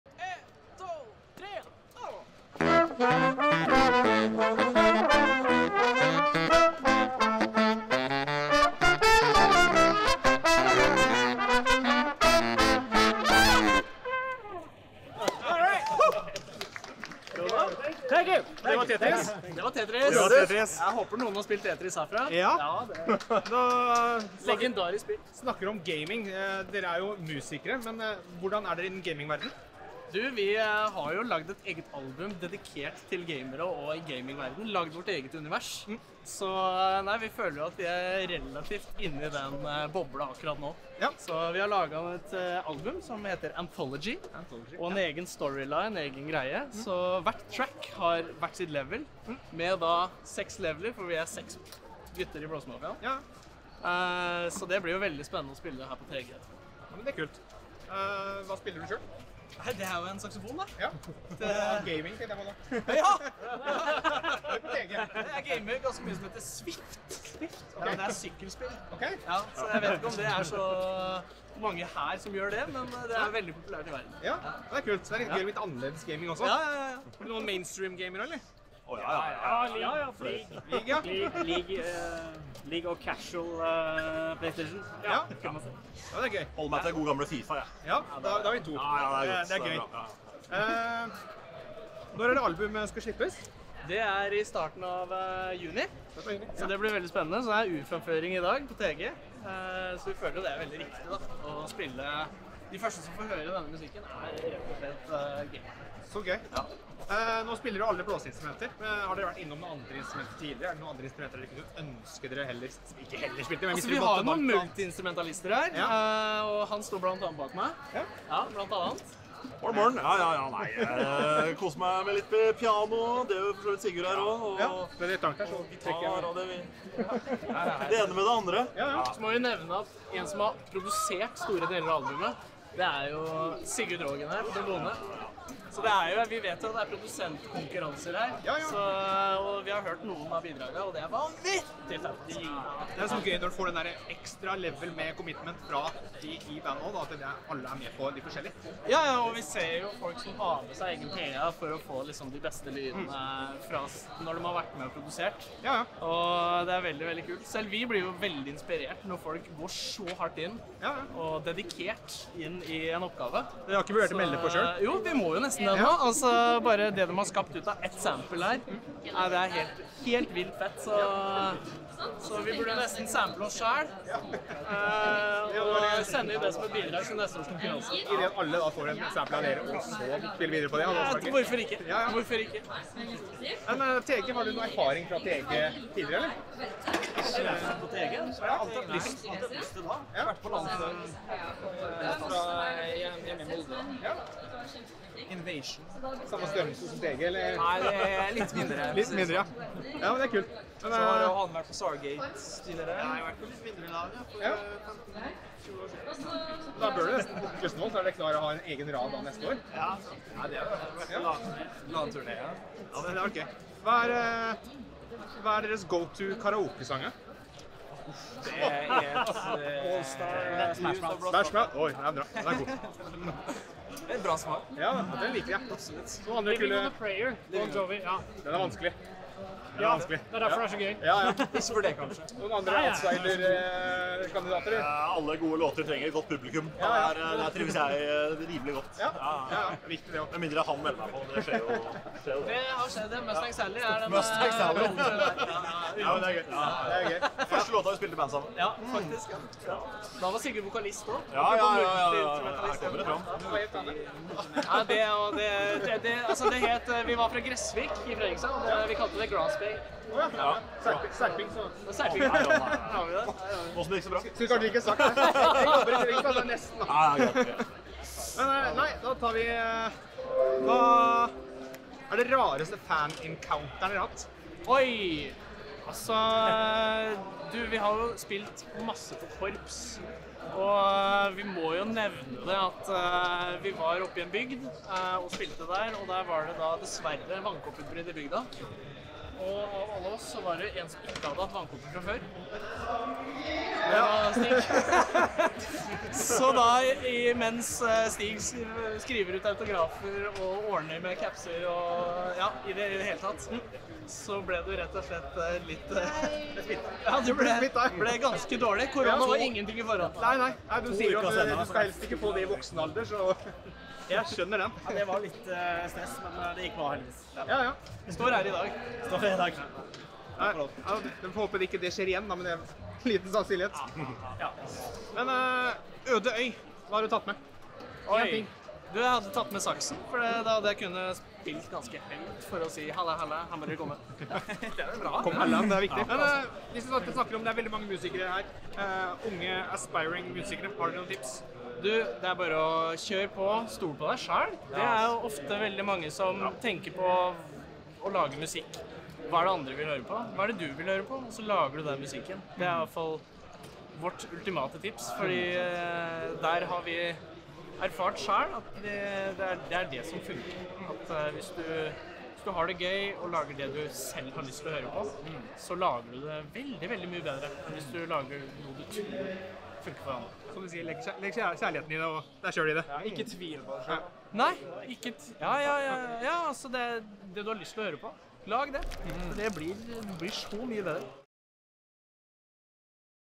TG23: Blåsemafian - intervju
Kim Intervjuer Blåsemafian før kveldens show!